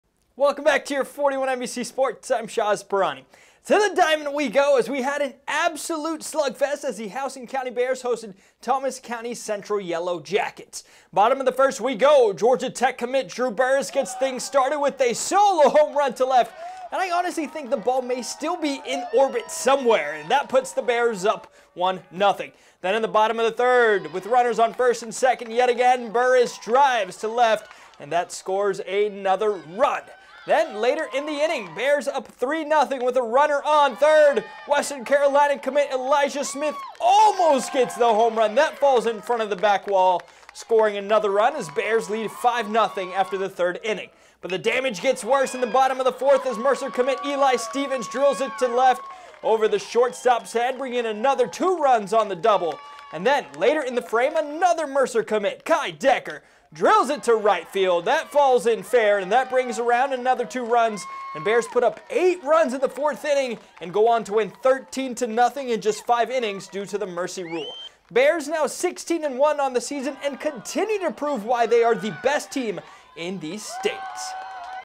Houston County dominates Thomas County Central on the diamond
WARNER ROBINS, Georgia (41NBC/WMGT) -- The Houston County Bears put on an offensive show dominating Thomas County Central 13-0 on the diamond.